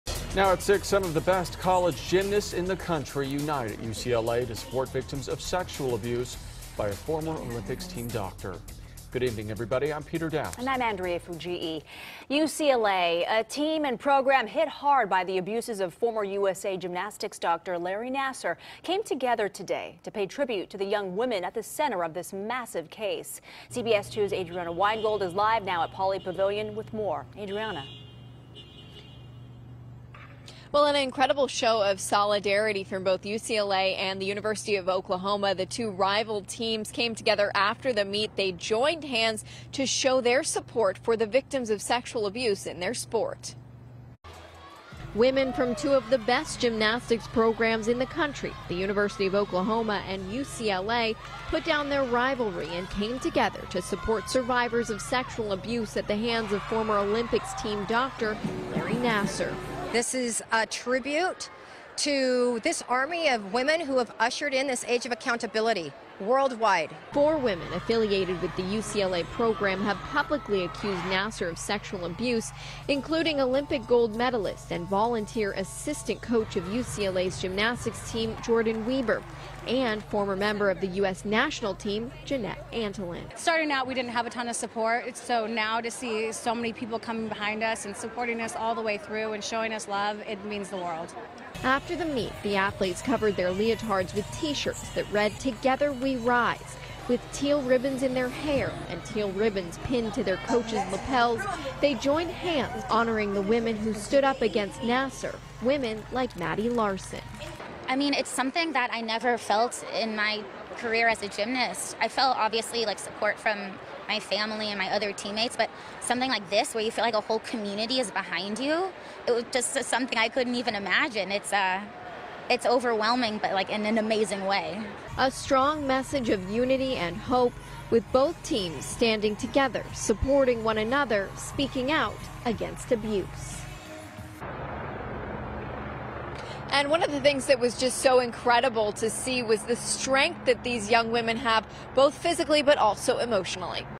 Gymnastics Teams Compete, Come Together In Support Of Nassar Sexual Abuse Victims
Women from two of the country's best gymnastics teams competed Sunday and came together to recognize their fellow gymnasts who spoke out against sexual assault in their sport. Adrianna Weingold reports.